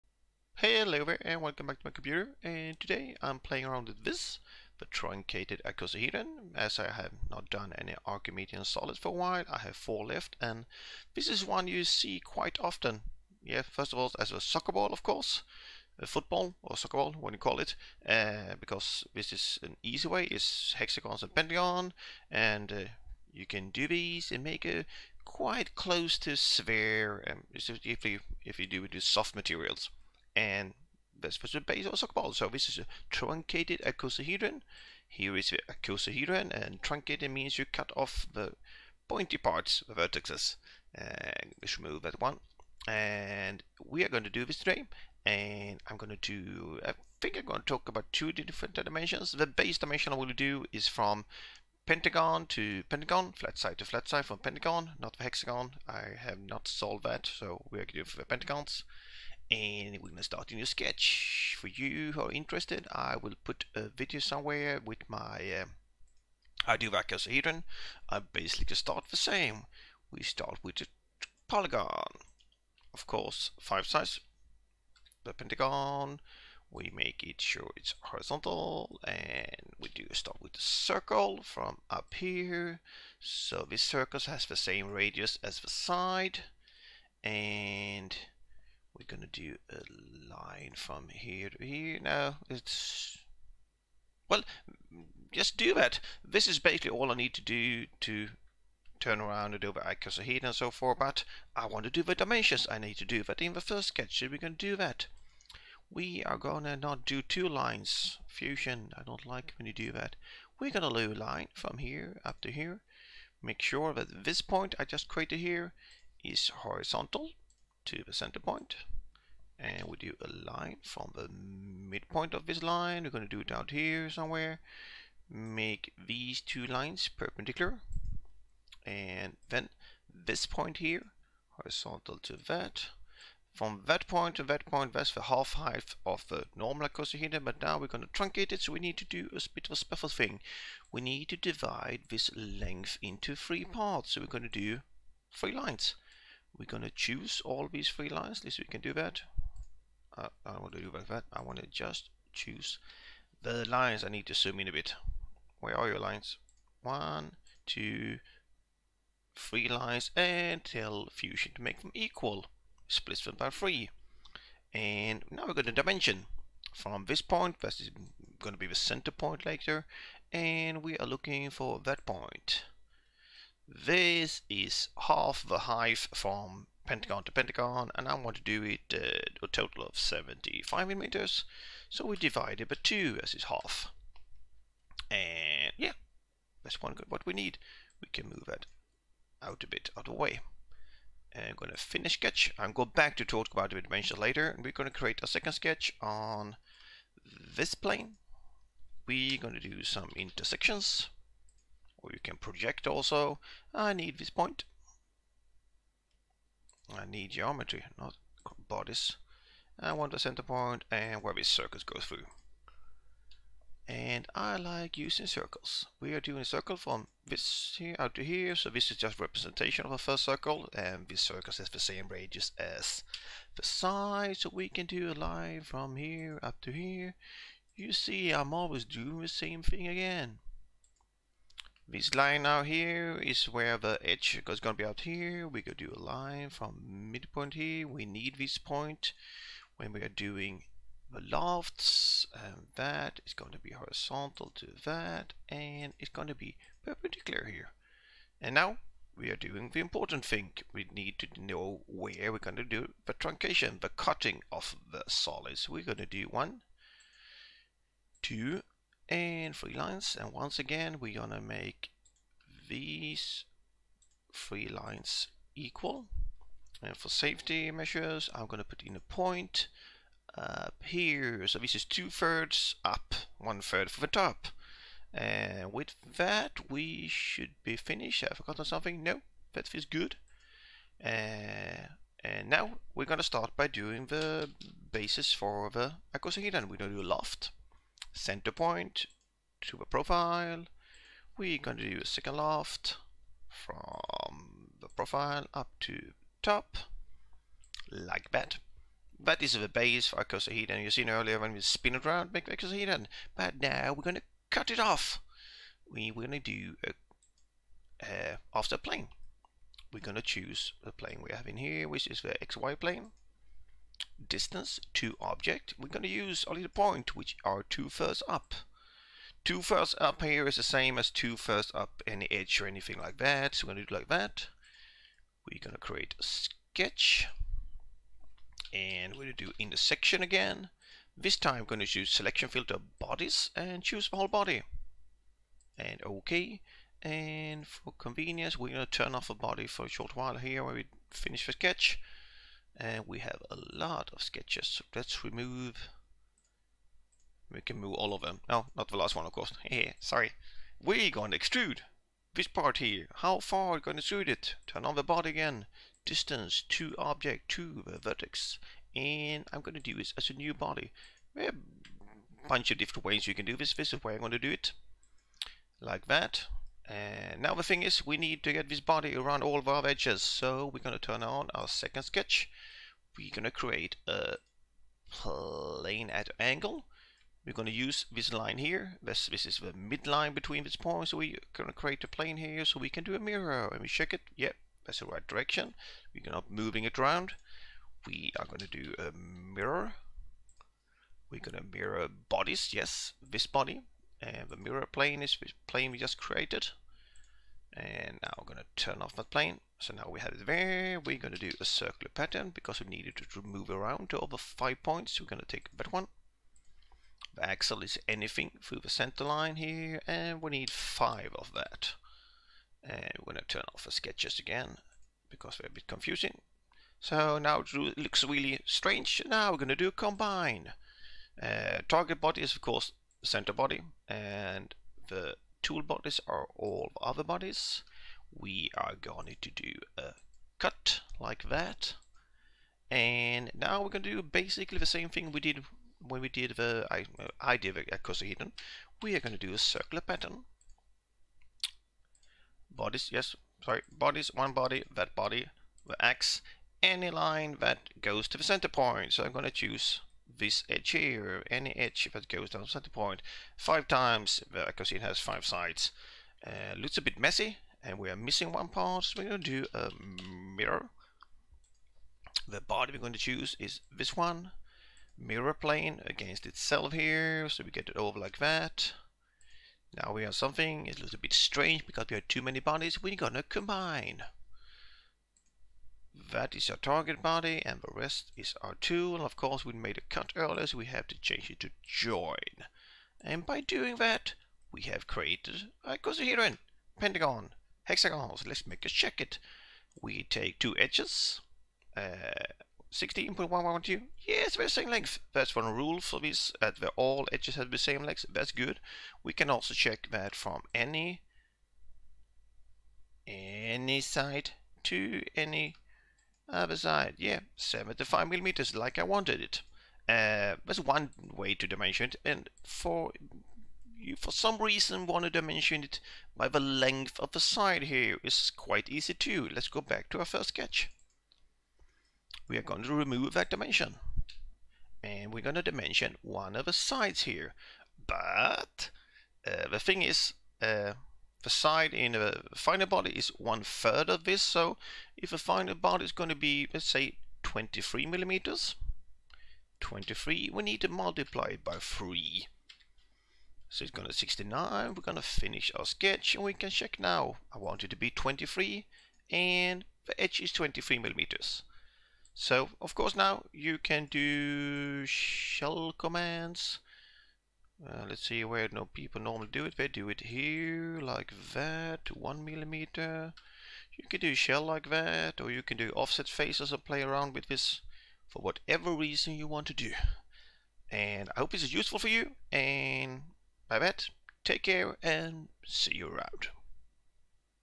Truncated Icosahedron (Soccer ball / Football) - Easy in Fusion 360 - No Math or Angles
This is a short video of me designing a Truncated Icosahedron (archimedean solid) in Fusion 360.
Using no math or angles, making the software do all the "hard" work.
The model is parametric in that there is only one dimension, then changing the dimension in the first sketch the final solid body updates.

Hope this is useful for you out there :)